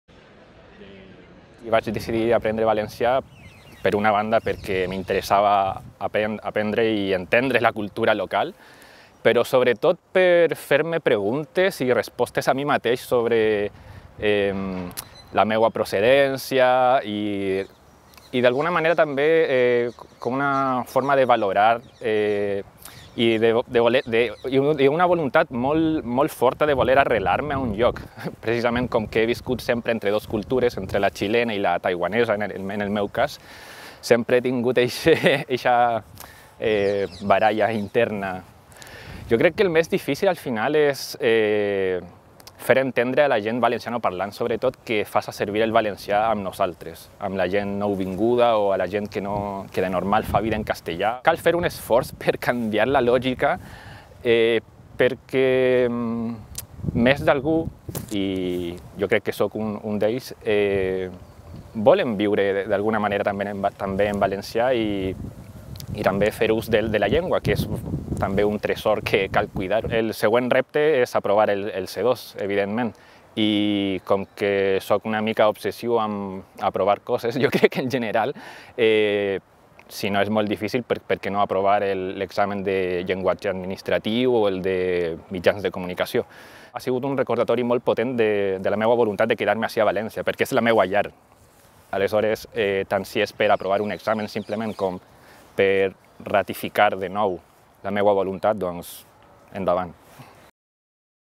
Com Chi-Hua Salinas ha après el valencià - Noticia @UPVTV, 23-11-2021
L’any 2014, Chi-Hua Salinas Sung (xilè – taiwanès) va arribar a València per a estudiar el Màster en Fotografia de la UPV.
Tres anys després, va decidir aprendre valencià amb l’ajuda del Servei de Promoció i Normalització Lingüística i ja té el certificat C1 de la Junta Qualificadora de Coneixements de Valencià.